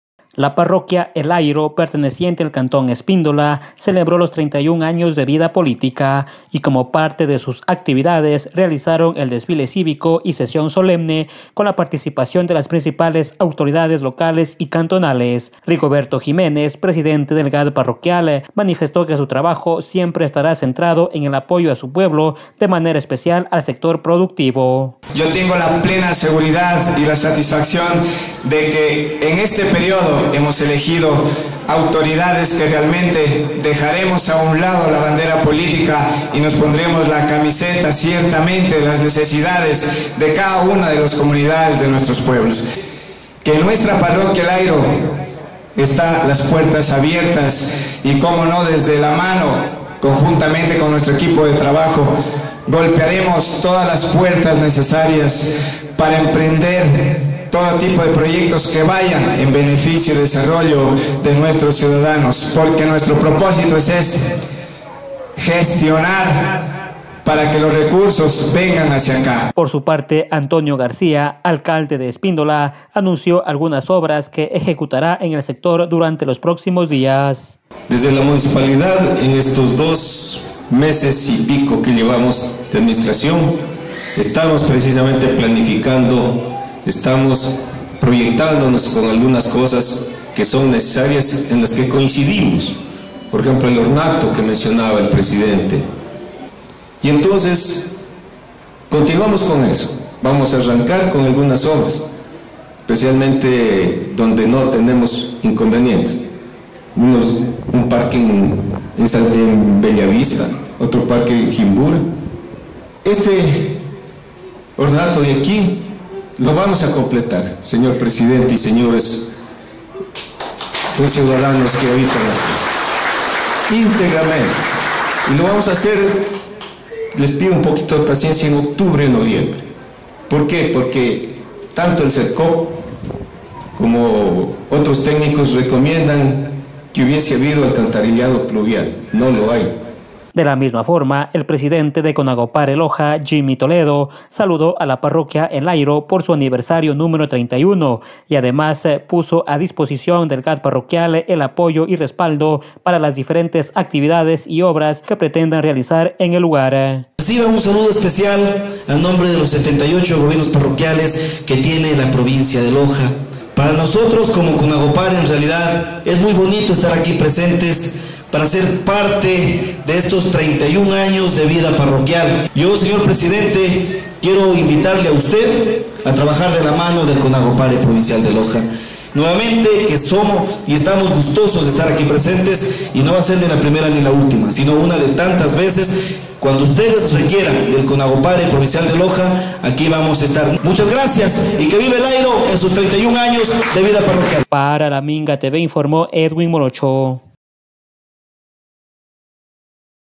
PARROQUIA EL AIRO
La parroquia El Airo, perteneciente al cantón Espíndola, realizó el desfile cívico y sesión solemne por cumplir 31 años de vida política, el evento contó con música, danzas y otras actividades.